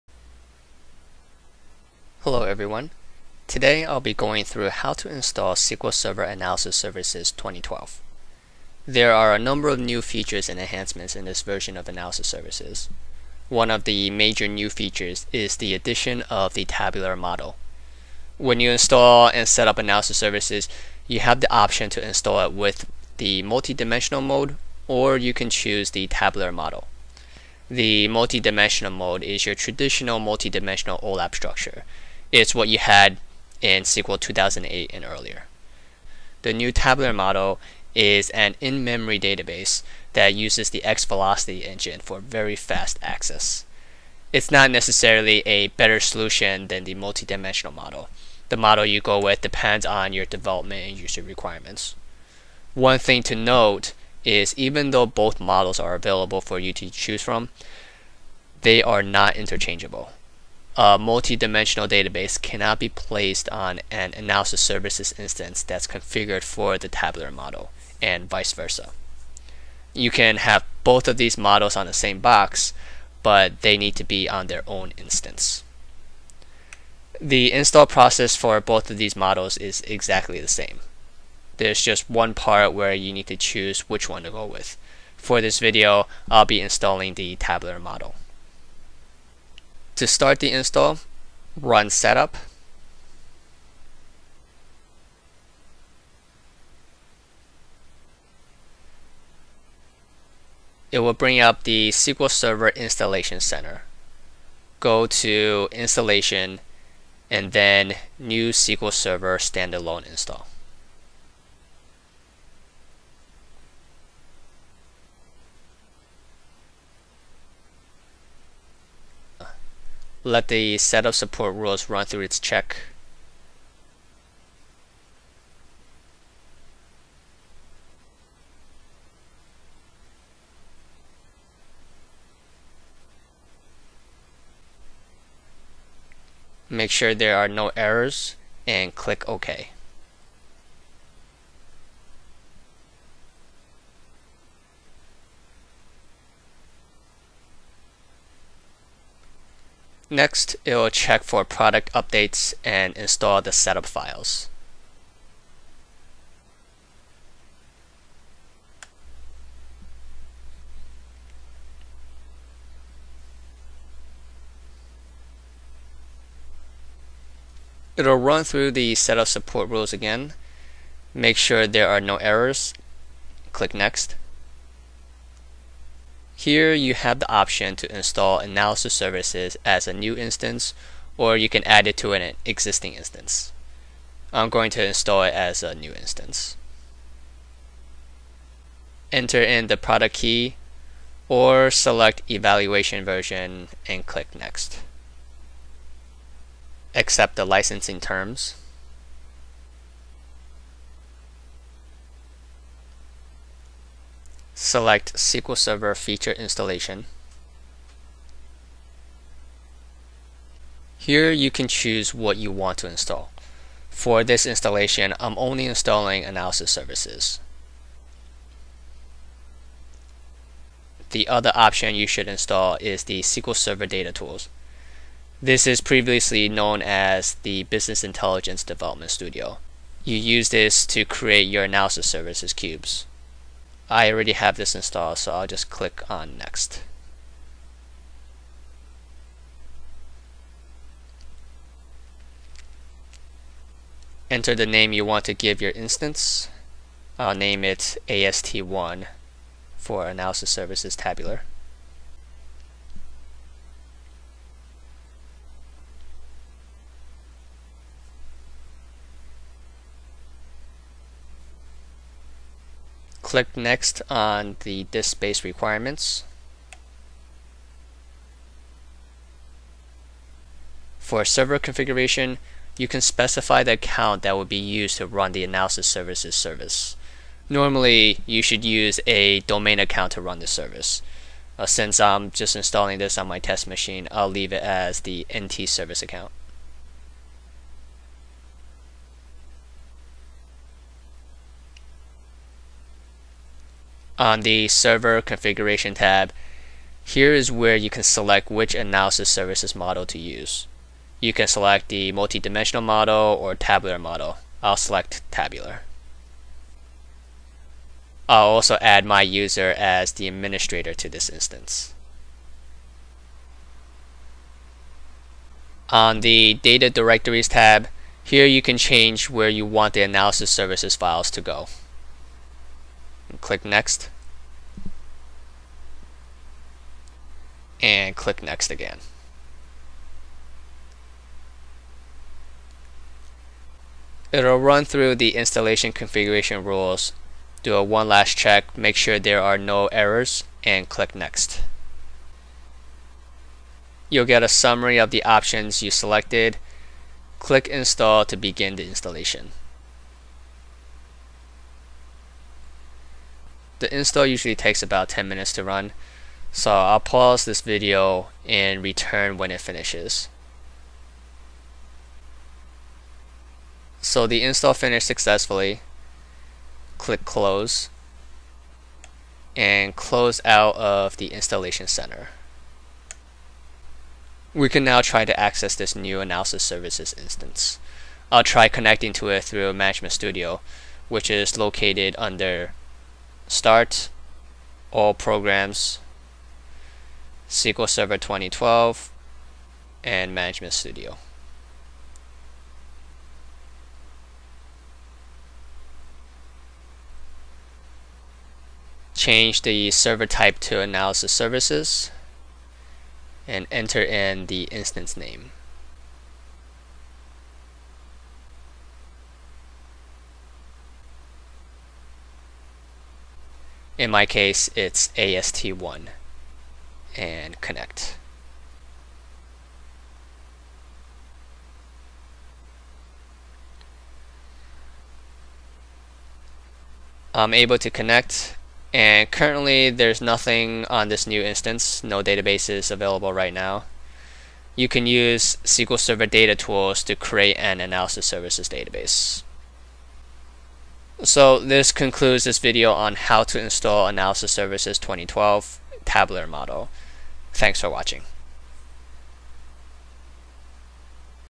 Installing Analysis Services 2012 - Tabular Model
This video walks you through how to install SQL Server Analysis Services 2012.

You can get the latest service pack or cumulative update for SQL Server and Analysis Services 2012 from: